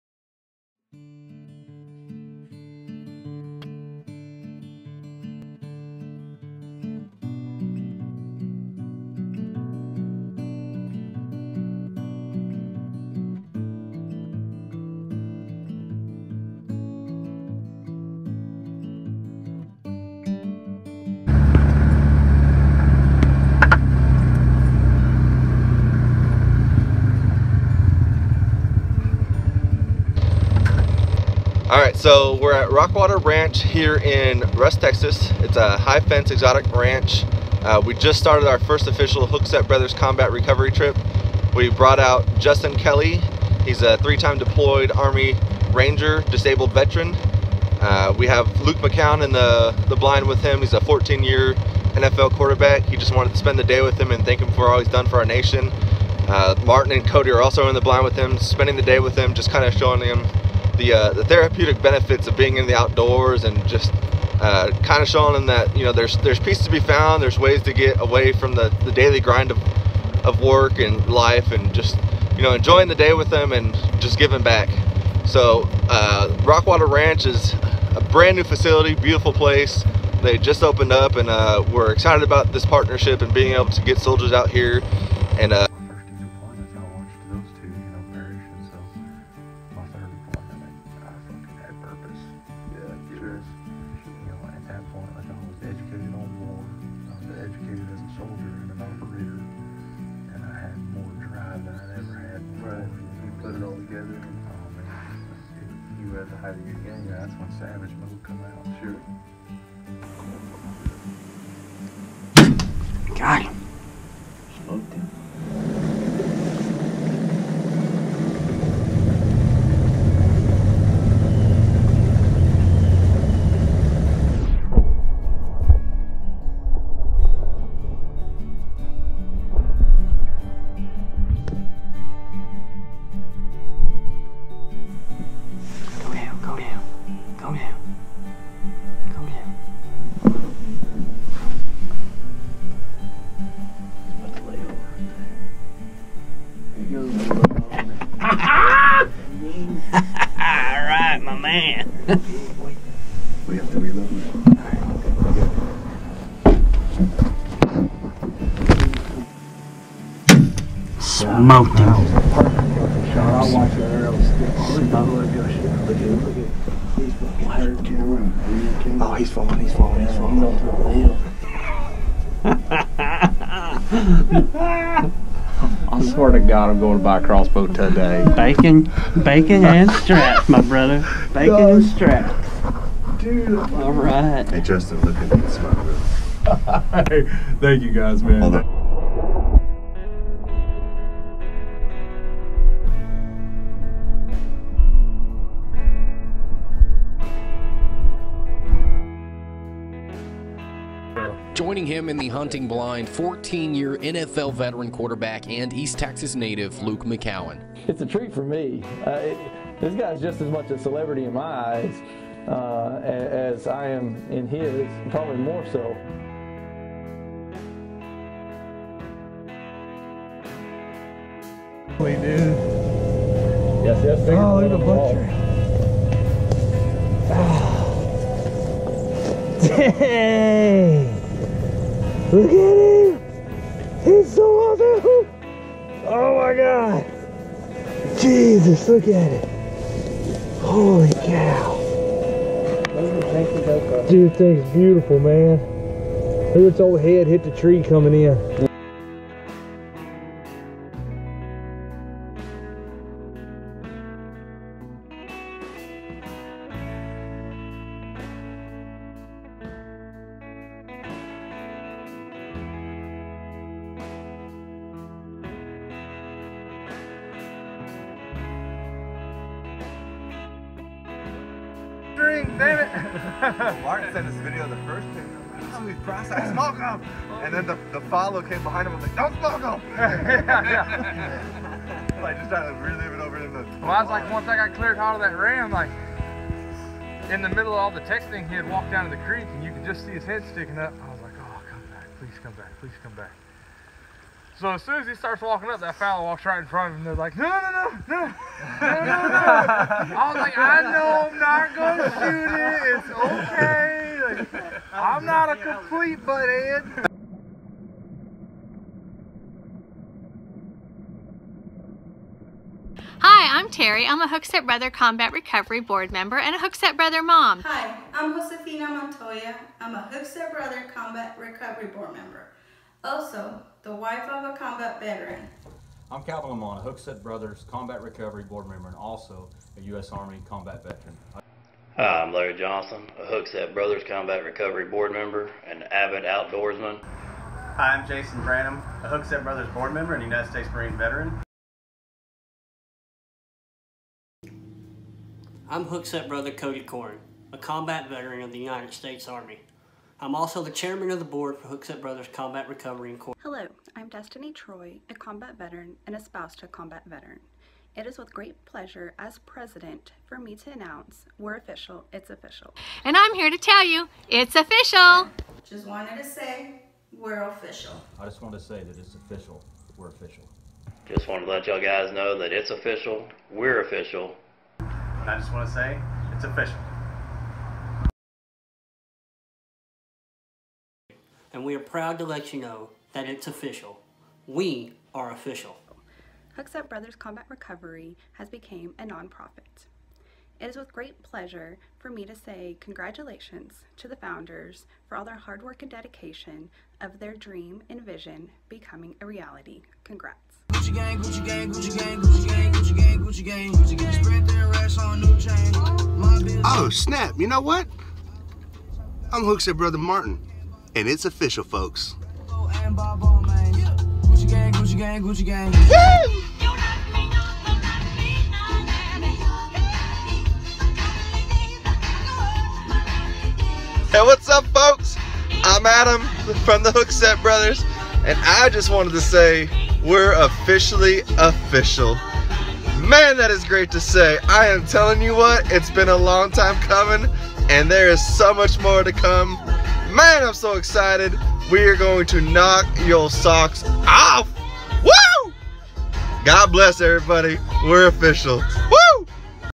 US Army Ranger Disabled Combat Veteran Justin Kelly Exotic Hunt with Hookset Brothers
Hookset Brothers Combat Recovery